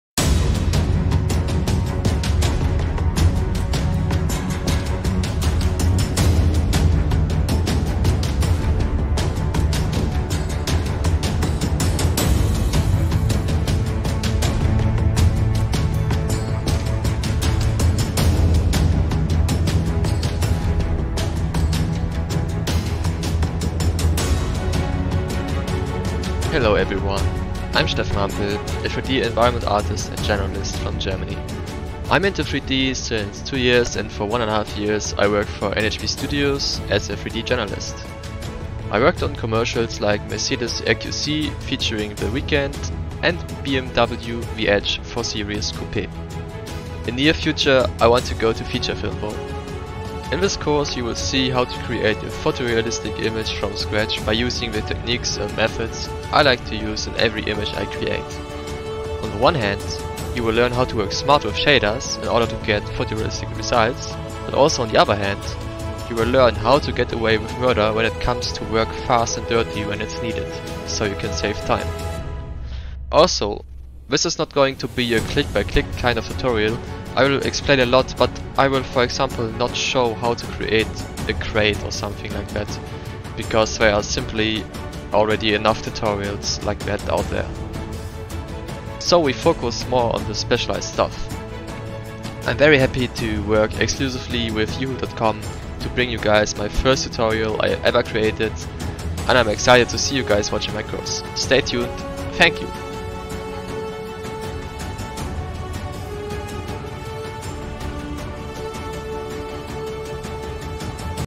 Creating a Photorealistic Japanese Alleyway with Maya & V-Ray Tutorial
In this tutorial series, you will learn steps on how to create photorealism, the basics of shading, and especially how to use your shaders smartly.